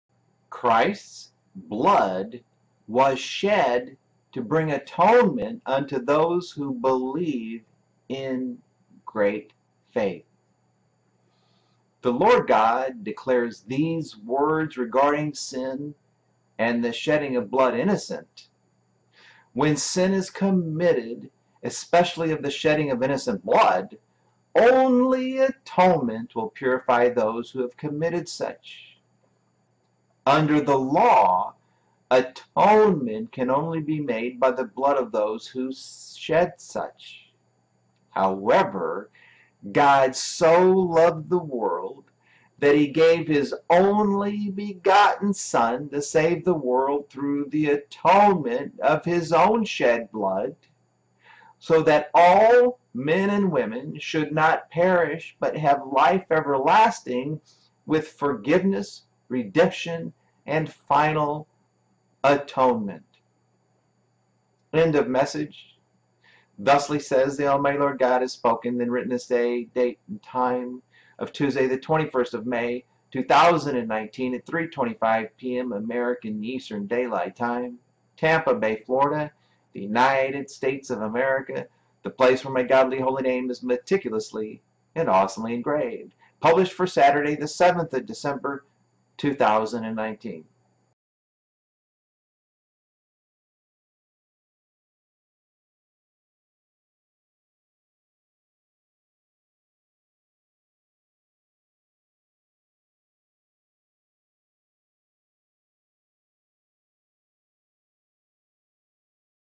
Christ’s Blood was Shed to Bring Atonement unto Those who Believe in Great Faith …
Published … for Saturday, the 7th of December 2019.

541.56---b.01